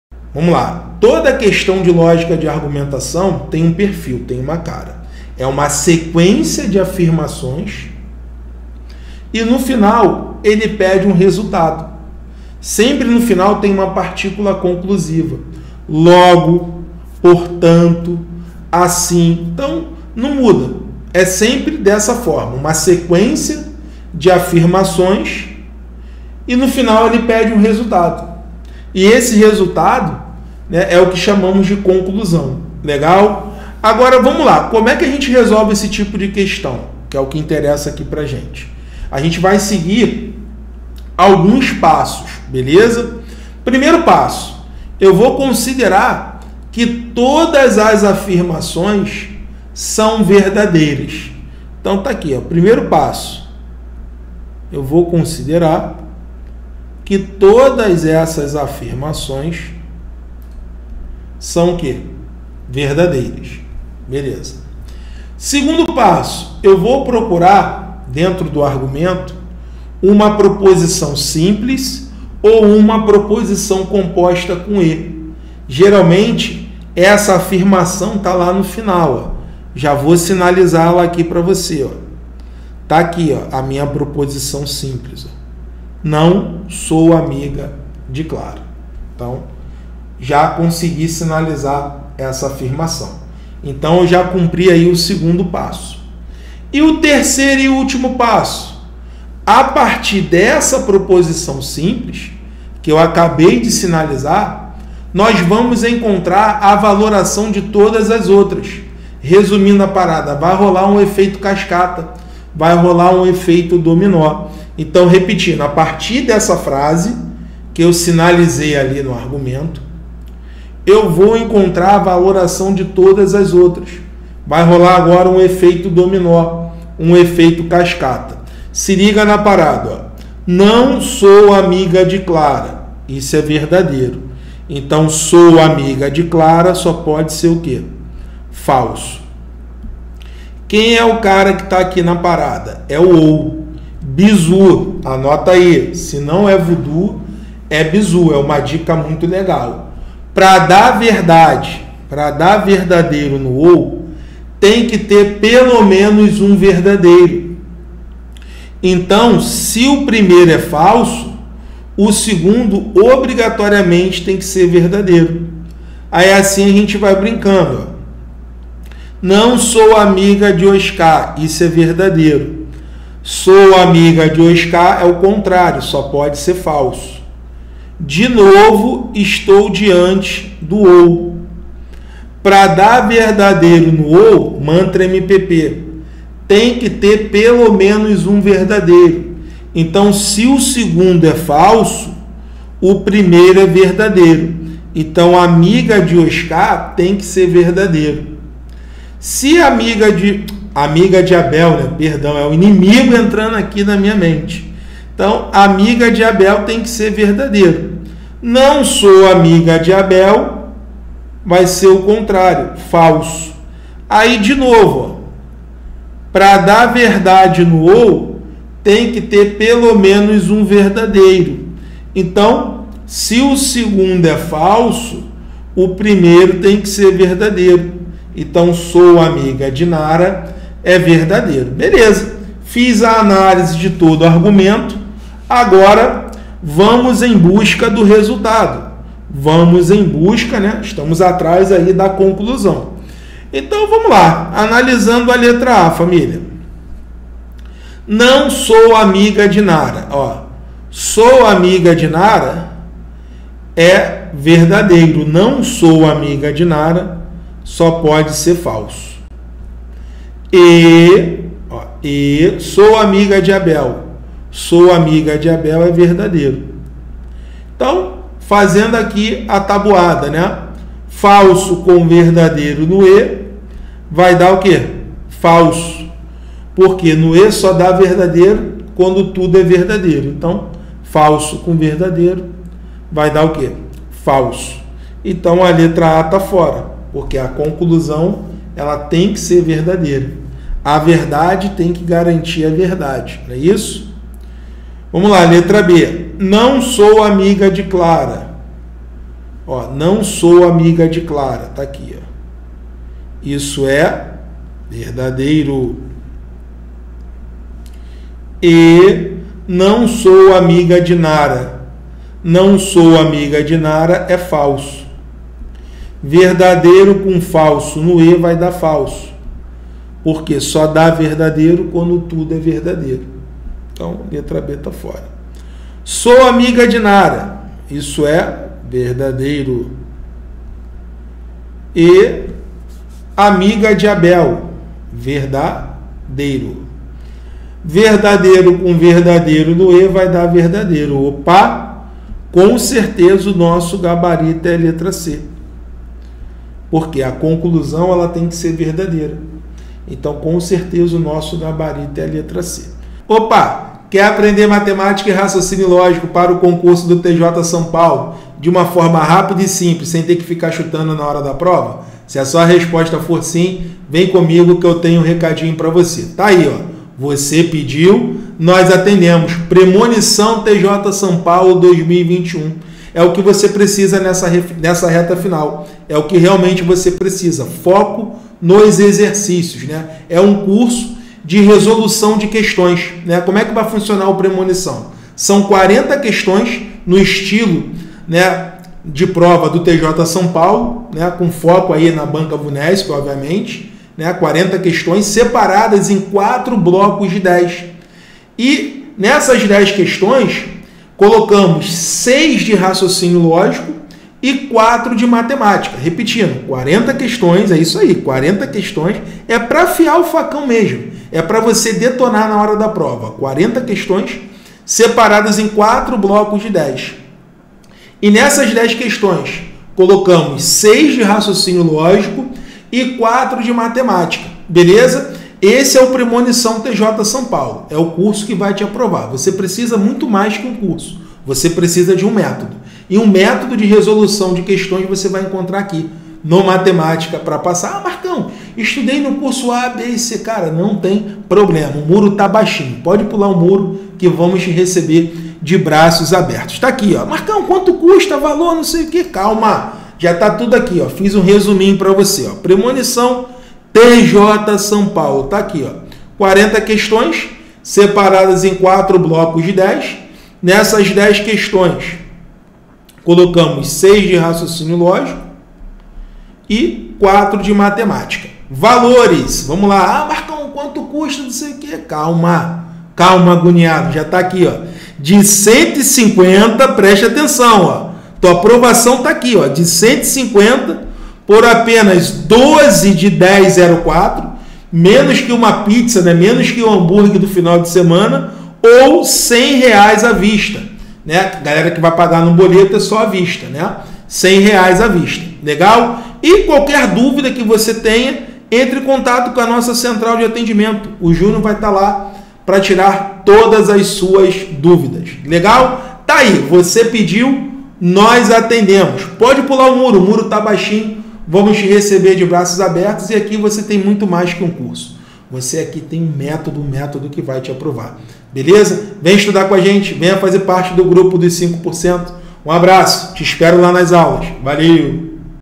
Raciocínio Lógico para TJ SP |Dica 61| Agora Ficou Fácil Lógica de Argumentação
Você não sabe Raciocínio Lógico para o TJ SP ?

Então você precisa aprender com esse vídeo onde nosso professor, Marcão, mostra Raciocínio Lógico para o TJ SP.

Você terá um passo a passo para aprender Raciocínio Lógico para o TJ SP e sair na frente de muitos concorrentes e mandar bem no concurso do TJ SP.

Agora basta dar o play no vídeo e aprender com o Método MPP Raciocínio Lógico para o TJ SP.